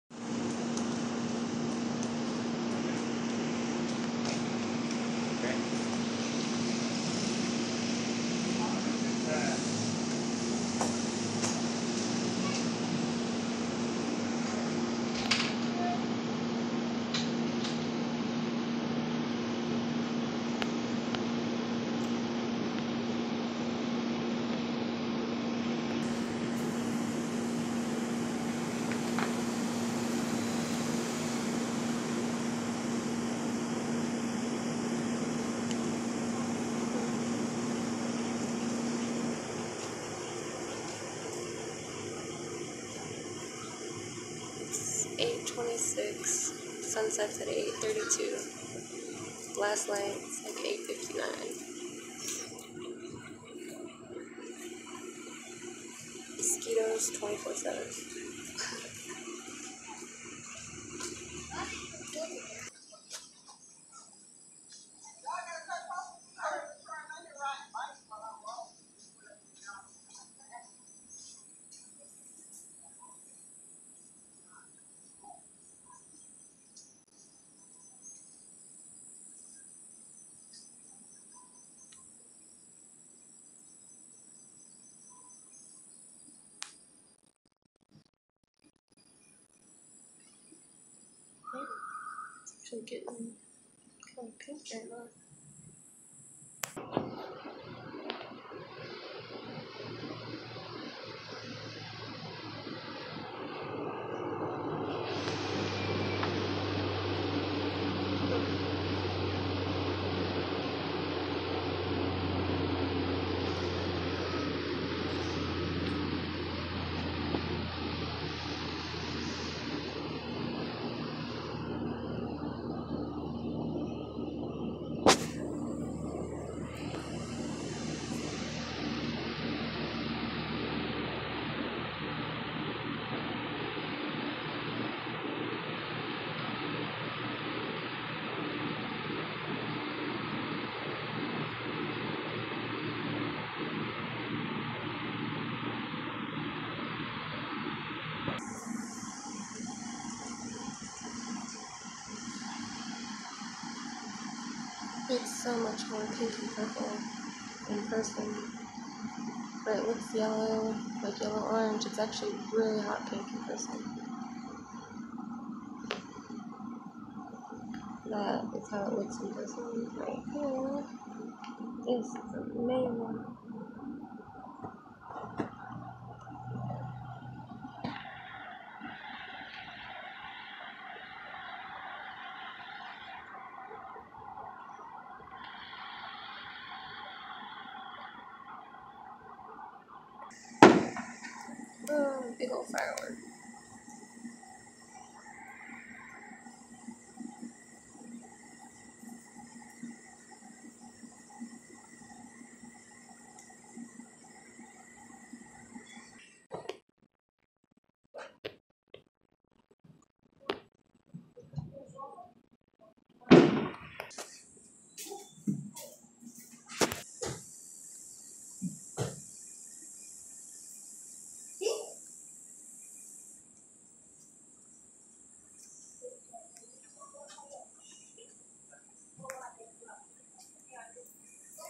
Pink & Purple Sunset!
Recorded at 8:35p on 7/5/17 from Jax, FL

*Sunset was at 8:32p
*Last Light was at 8:59
*Mosquitoes↝24/7

I started recording this video less than 10 mins before Sunset and finished recording about 5 mins after Sunset, in hopes of catching the actual Orb of the Sun, dipping below the Horizon. The good 'ol Cloud Wall along with other "wrangled in" Clouds blocked my view, once again. Nevertheless an amazing show at Sunset! The Moon also decided to show its face this evening, as well!

NOTE:  Always check the quality of the videos before you play them, as YouTube likes to automatically post them in lowest quality settings.

Feedback always welcome..